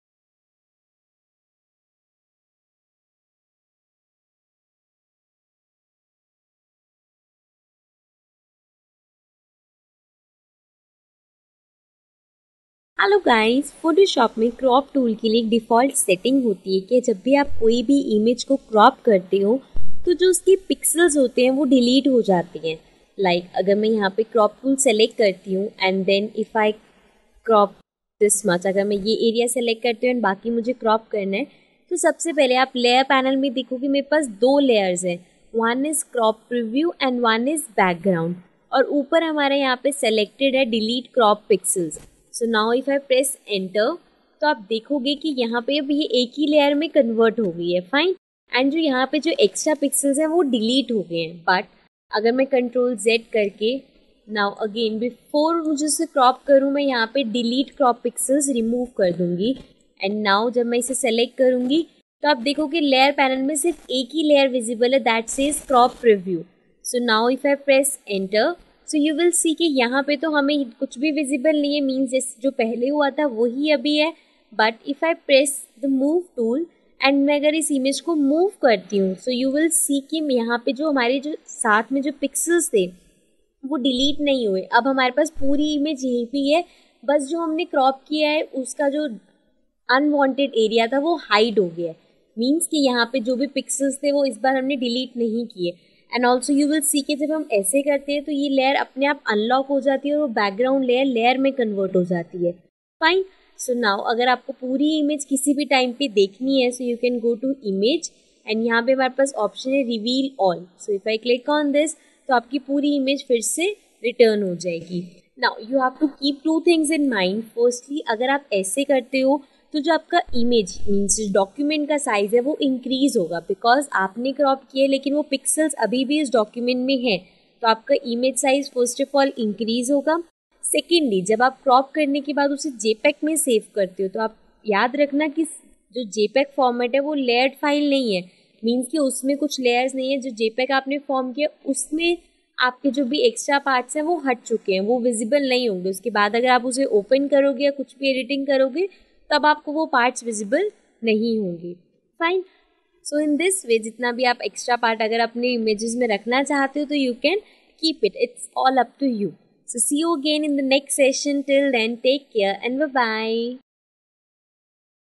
Exploring Different Crop Options in Photoshop || Chapter 3 | Video 3
In this tutorial we will be Exploring different Crop options. To view the photoshop beginner tutorials visit evidyarthi.in